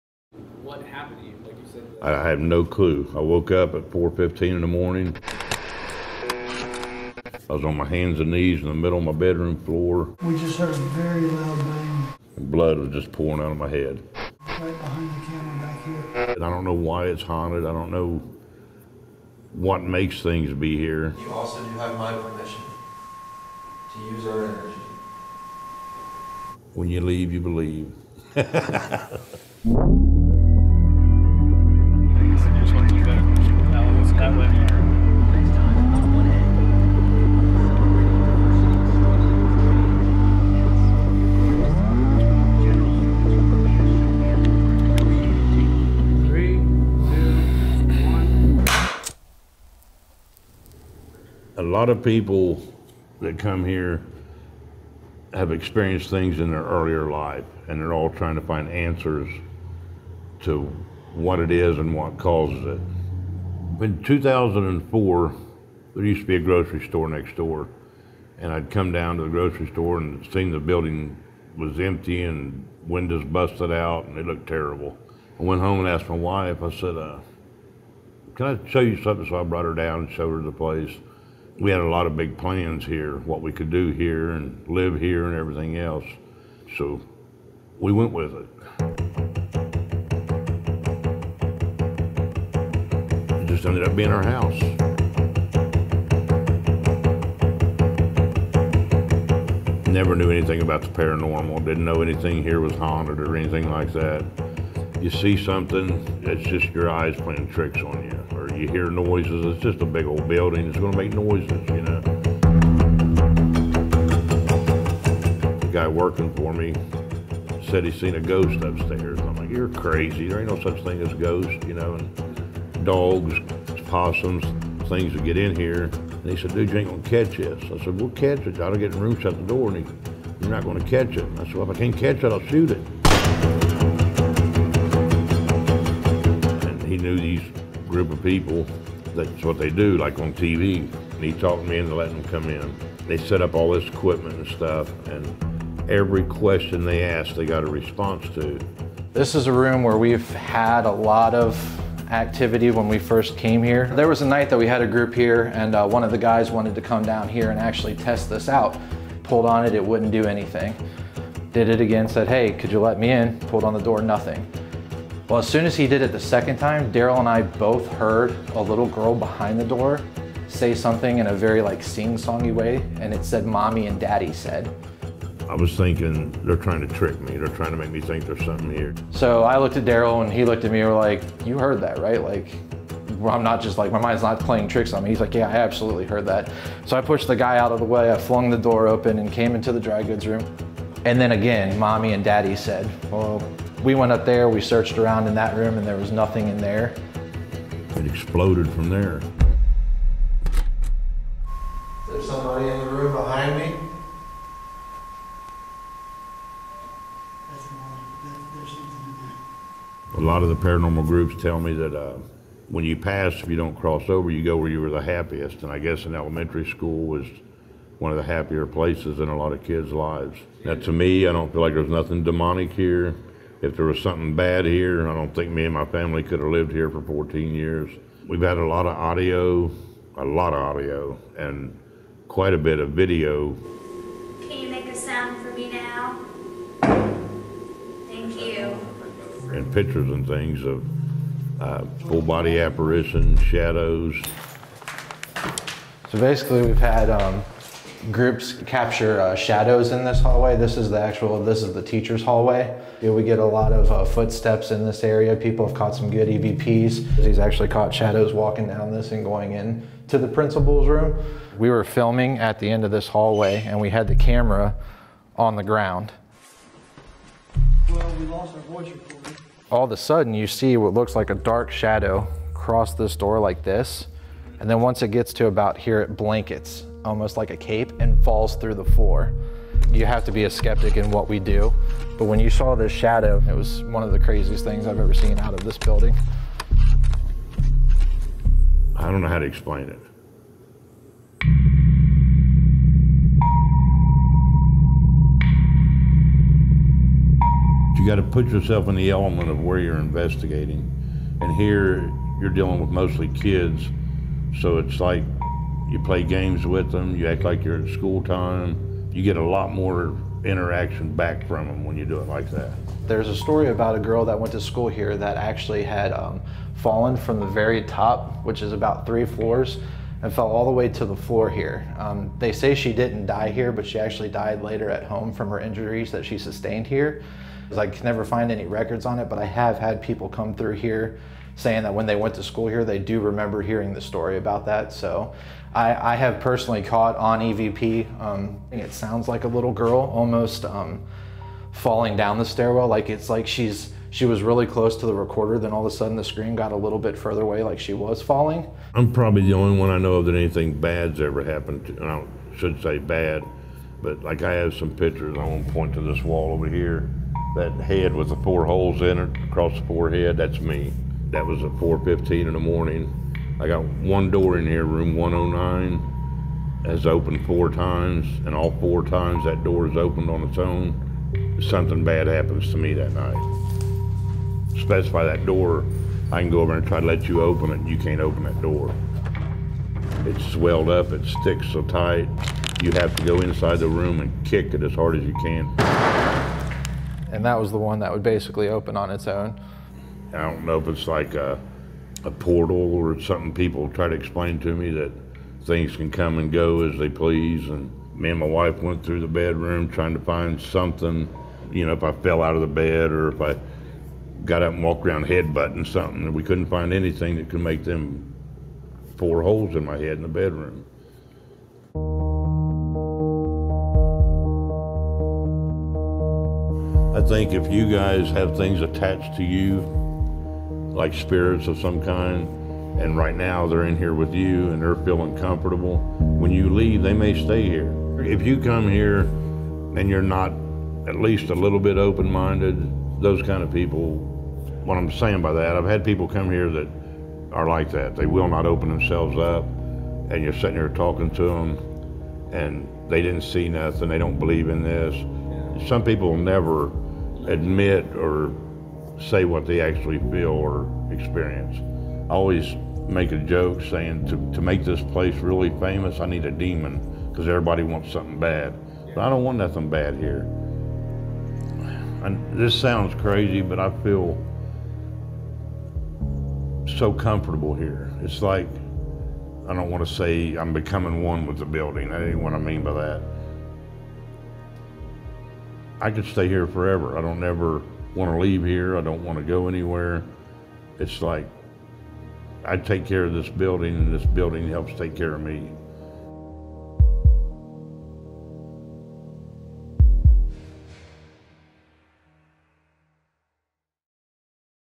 An Inside Look At Poasttown Haunted School
Do you believe? Meet the man living in a haunted, abandoned school in Middletown, Ohio.